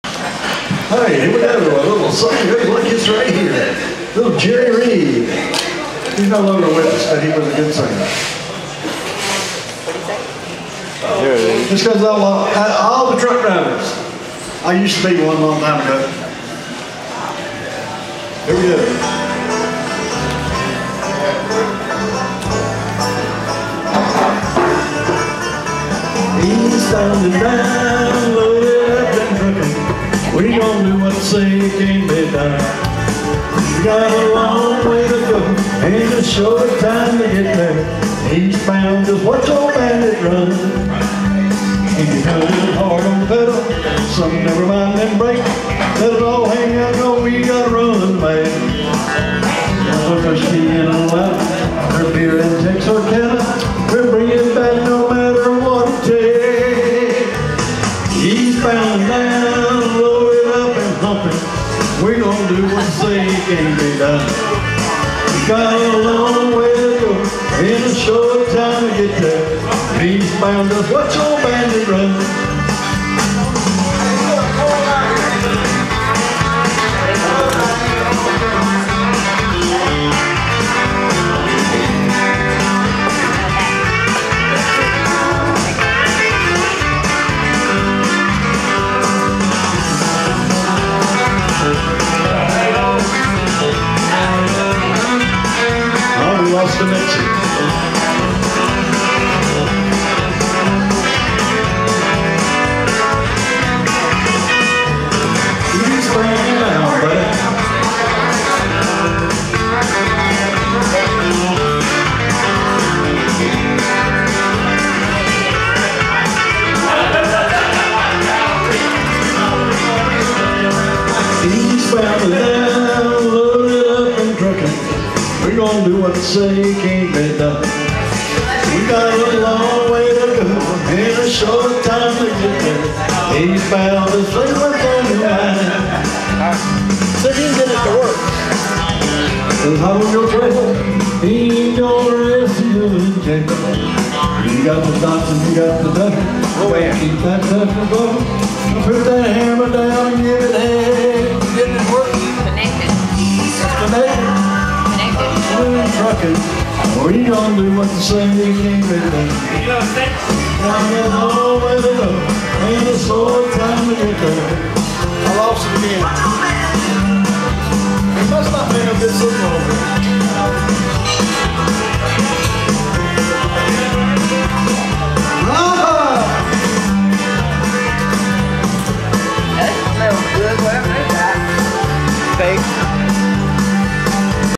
Jimmy Joe at El Patron..10-28-2016
Jimmy Joe karaoke at El Patron every Friday nights.7-11 pm